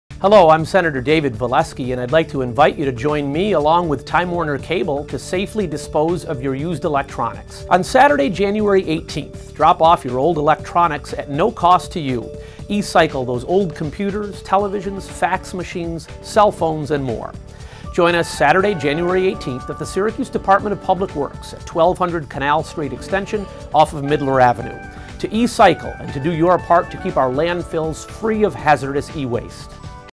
Electronics recycling with Senator Dave Valesky - Jan. 18
Time Warner Cable is proud to join NYS Senator Dave Valesky in hosting a free electronics recycling event on Saturday, Jan. 18, at the Syracuse Department of Public Works. Hours are from 10 a.m. to 3 p.m.